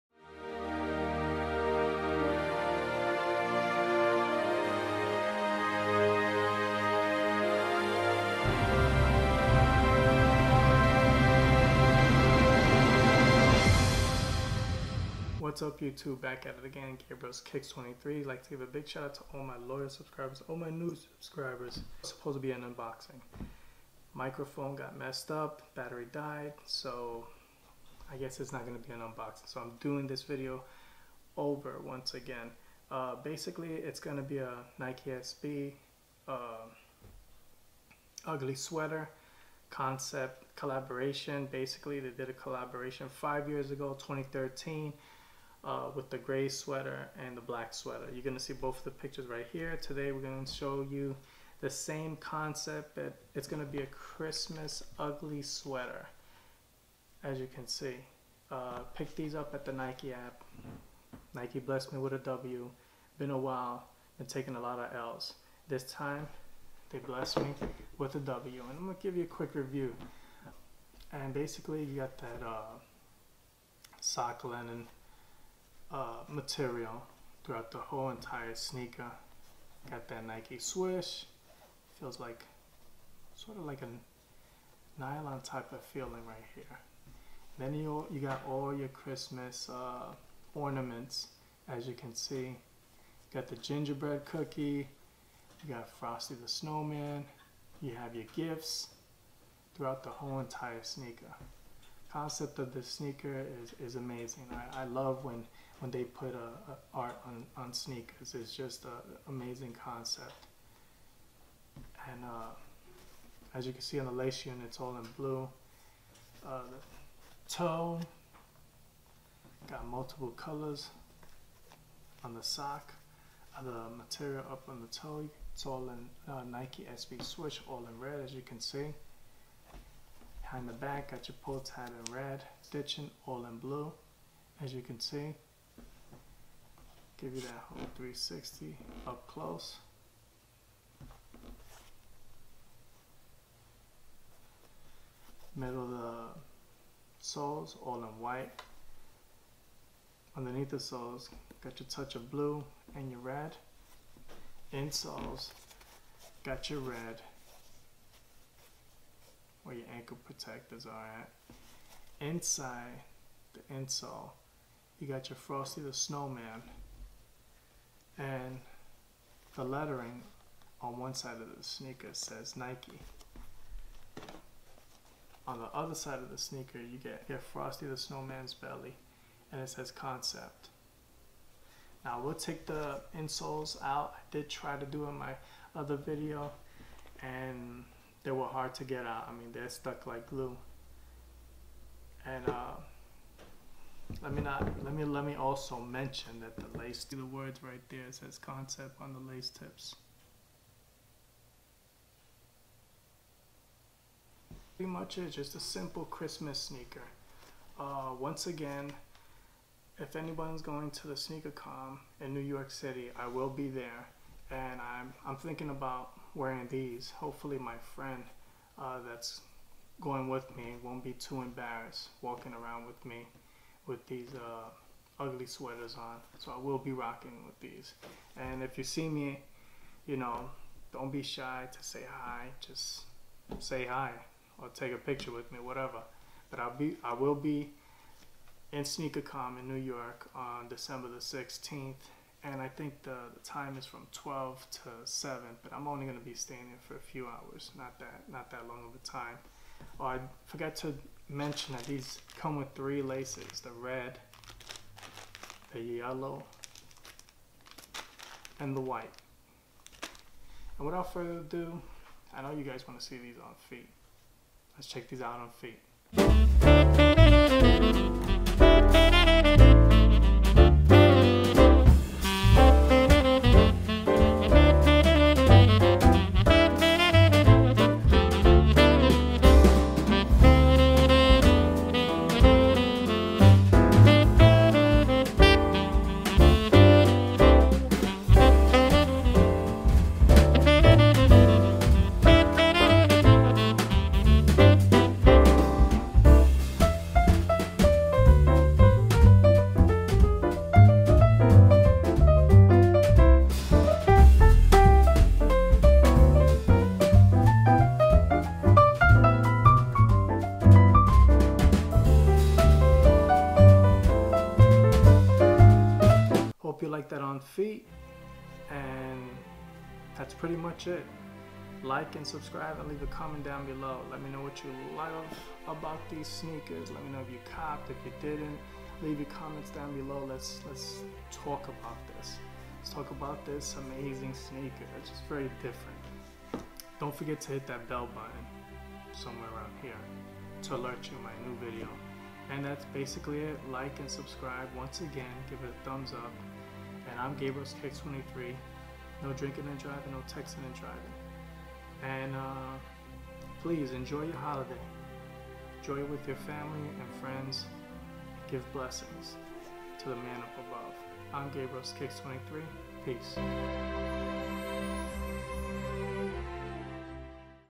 CONCEPTS X NIKE SB DUNK HIGH UGLY XMAS SWEATER/REVIEW & ON FEET
Amazing sneaker , concepts collaboration  with nike sb was on point creating such an amazing sneaker especially on a holiday , full review and on feet , hope you enjoy the video , please don't forget to hit that like /comment and subscribe , peace and blessings to you all , thank you for watching